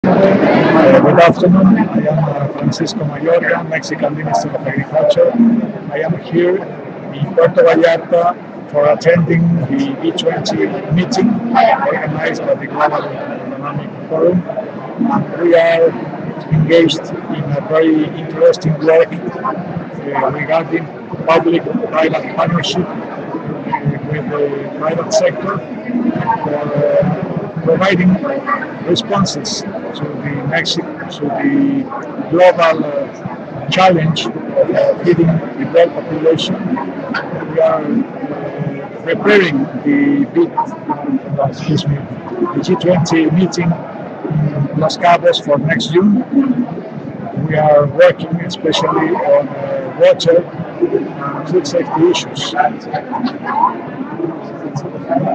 Latin America 2012- Francisco Javier Mayorga Castañeda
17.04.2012

Francisco Javier Mayorga Castañeda, Secretary of Agriculture, Livestock, Rural Development, Fisheries and Food of Mexico, joins the Ask a Leader series at the World Economic Forum on Latin America held in Puerto Vallarta, Mexico 16-18 April 2012.